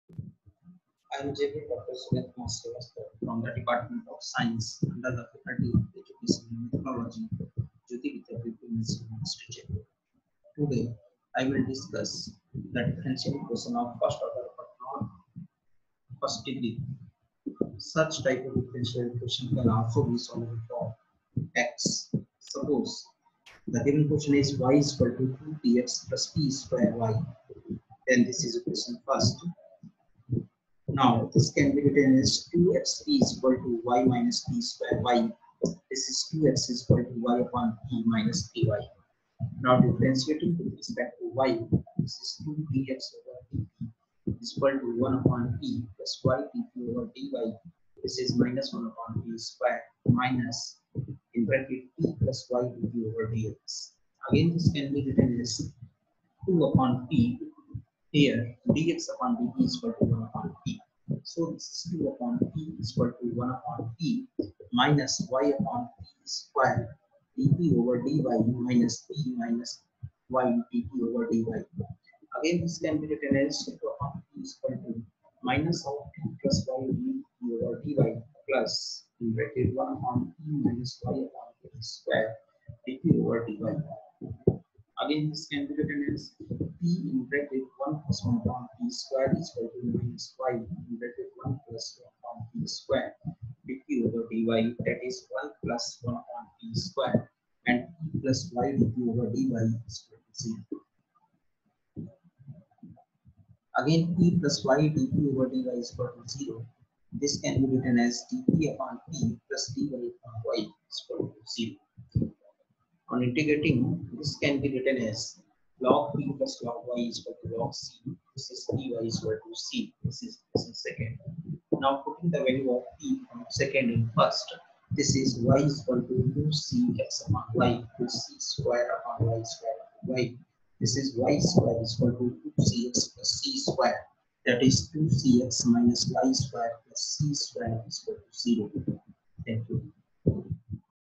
Equation solving for x
Faculty name - Jvn Dr Surendra kumar Srivastava
Dept. Of Science
 Faculty of Education and Methodology
Jayoti vidyapeeth womens university jaipur
Topic - Equation solving for x
Course – Differential Equation
Class- B.Sc/B.Sc-B.Ed IV sem